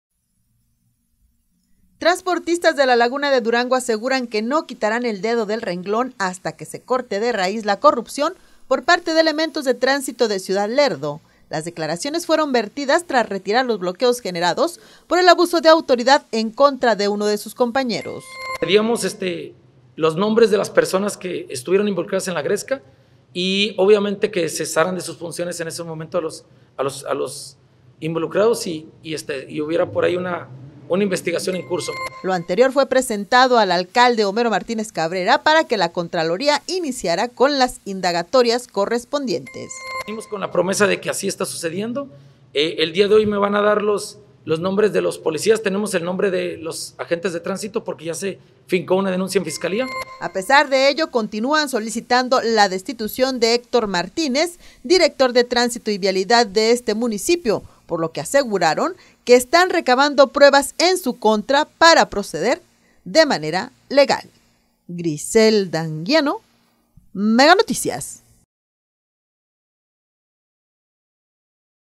Transportistas aseguran que no descansarán
→ Transportistas aseguran que no descansarán hasta que se elimine la corrupción en agentes de tránsito de Lerdo.